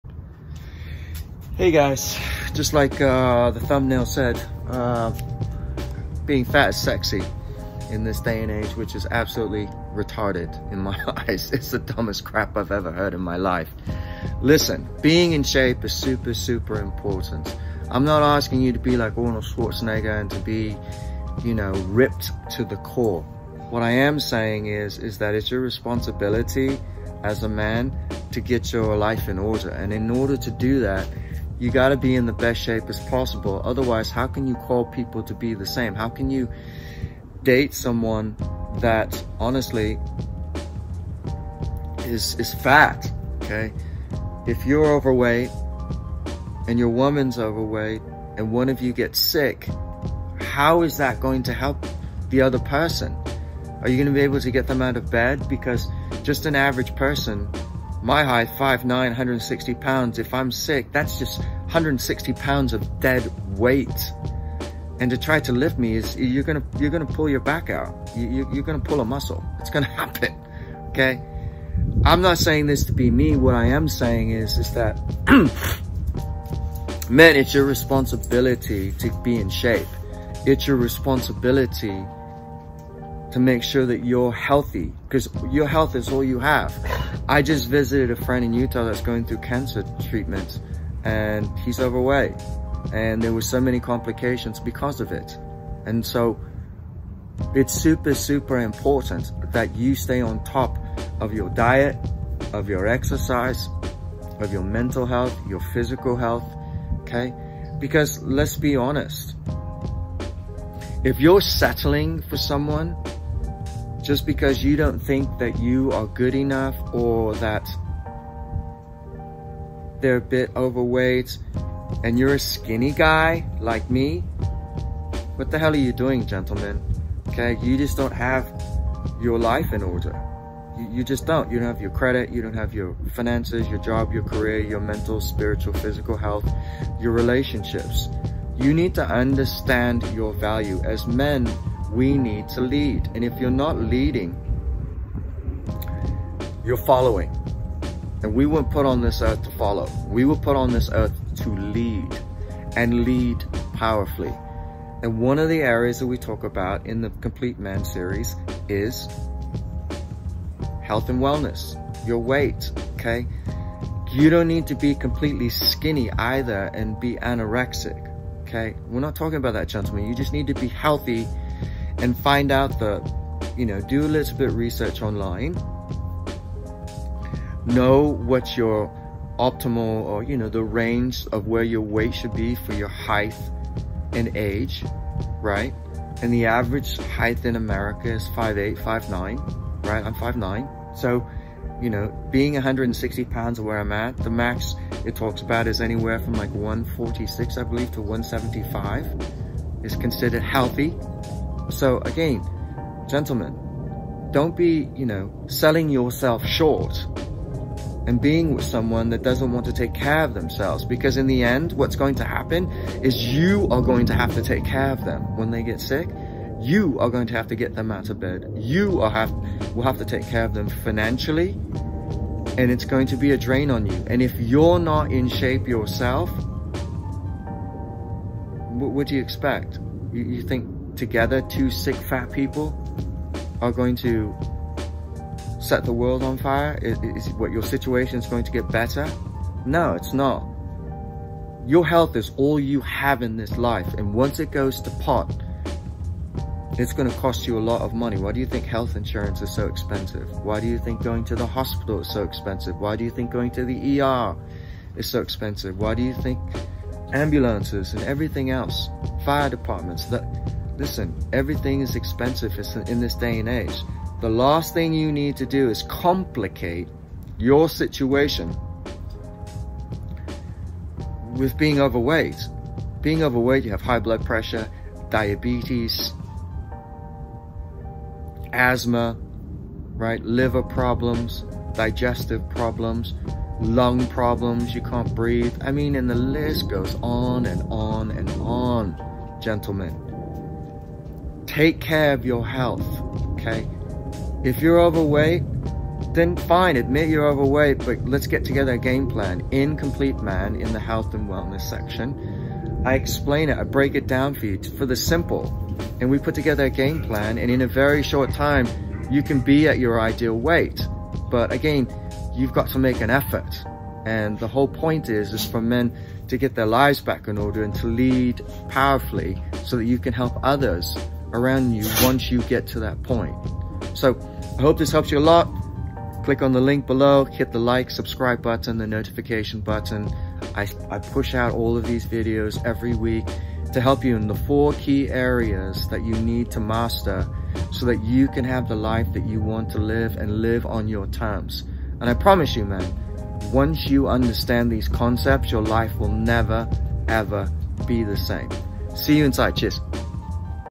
Being Fat Is The New Sexy
Men being overweight is not great & safe for your mental & physical health. It's time to step up & own up.
You can't work and function at your maximum with clarity and energy.
Learn how to be in the best shape you've been in: physically, mentally, financially, personal credit and much more.
and join the Complete Man Revolution.